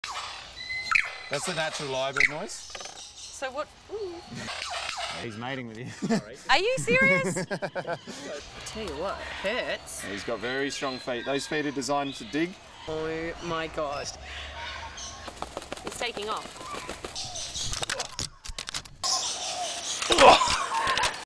Lyrebird Adelaide Zoo
Catherine Ellis, Behind the News: some quick highlights of our film shoot with 'chook' the lyrebird at Adelaide Zoo!